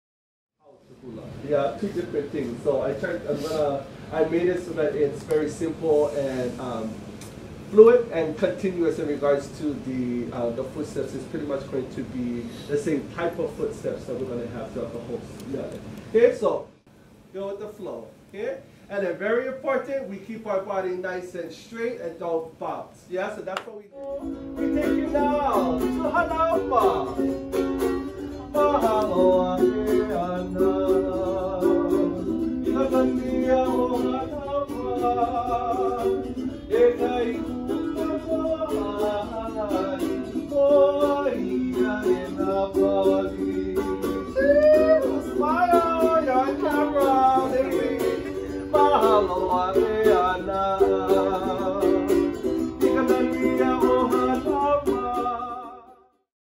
Hula 'Auana Mali'uka'ai Workshop
Session two of the mali'uka'ai cultural workshop series. Hula 'auana by kumu Kalani Flores.

Edited by: Toby York